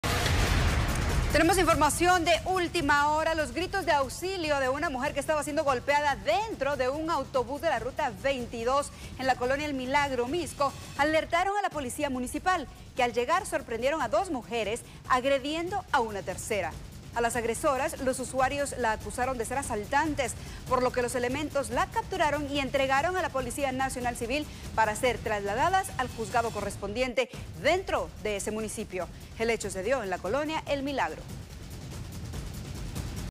Atacan a mujer en bus de la Ruta 22
En el transcurso de esta tarde, agentes de la Policía Municipal sorprendieron a dos mujeres agrediendo a una tercera en un autobús de la Ruta 22 en la Colonia El Milagro, Mixco. Efectivos policiales lograron capturar a dos presuntas delincuentes en la Colonia El Milagro